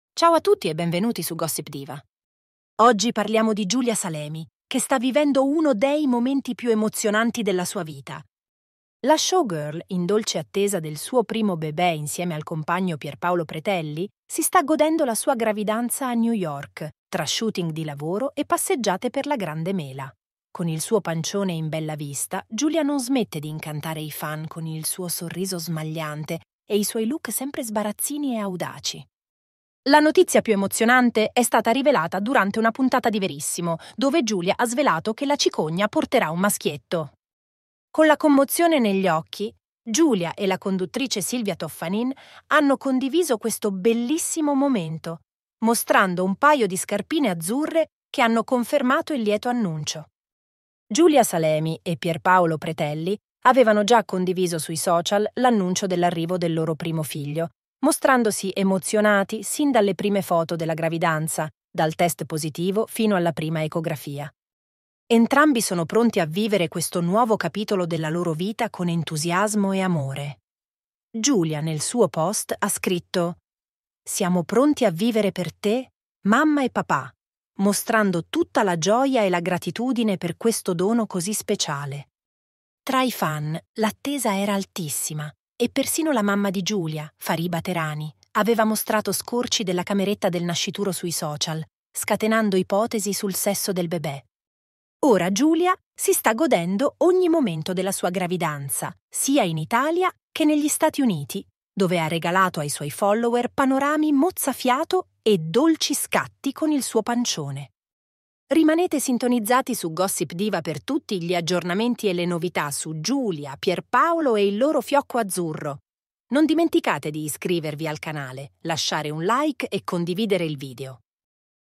"Giulia Salemi a New York: Felicità e Fiocco Azzurro in Arrivo!"
Ciao a tutti, e benvenuti su Gossip Diva! Oggi abbiamo delle bellissime novità su Giulia Salemi, che si sta godendo la sua gravidanza a New York! In dolce attesa del suo primo figlio con Pierpaolo Pretelli, Giulia ha annunciato durante *Verissimo* che la cicogna porterà un maschietto! In questo video vi sveleremo tutti i dettagli sulla sua emozionante avventura, dai panorami mozzafiato della Grande Mela ai momenti di tenerezza con il pancione.